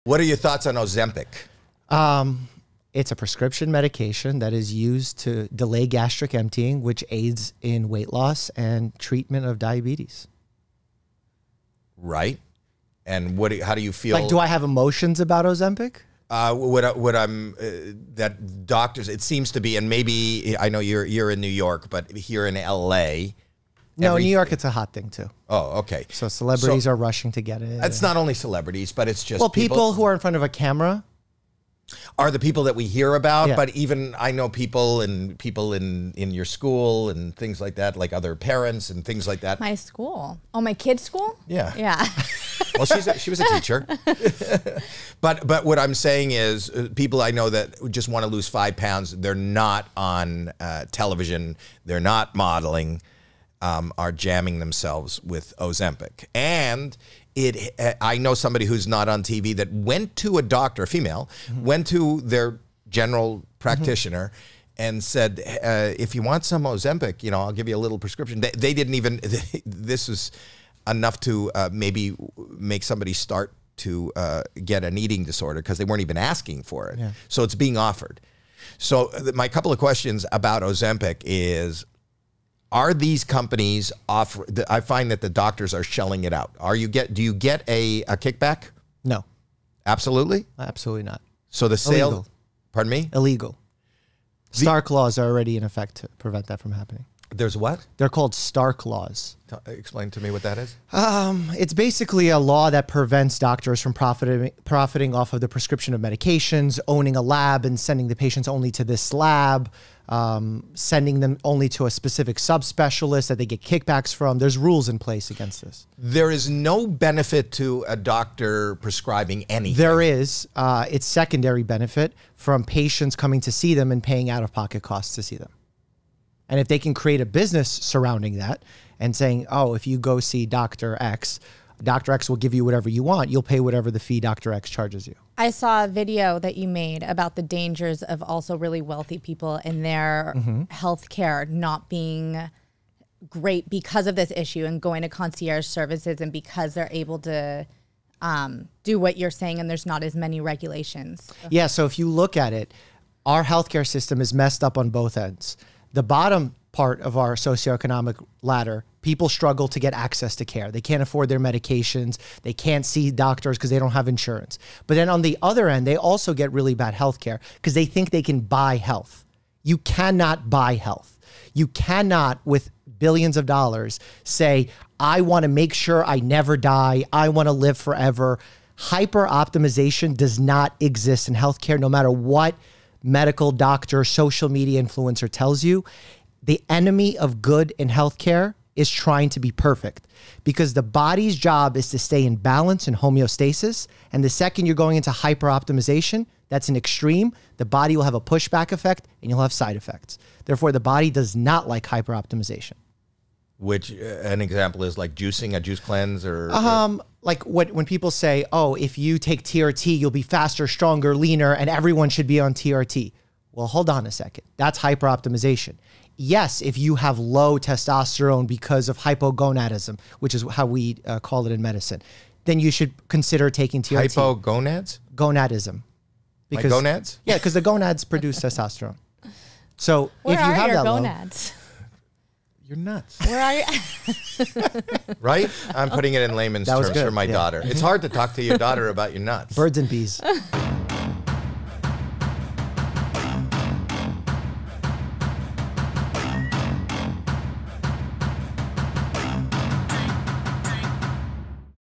Doctor Mike Explains Ozempic, The Controversial Weight Loss Drug
Doctor Mike, People Magazines Sexiest Doctor Alive explains how people are misusing the diabetic drug Ozempic for weight loss.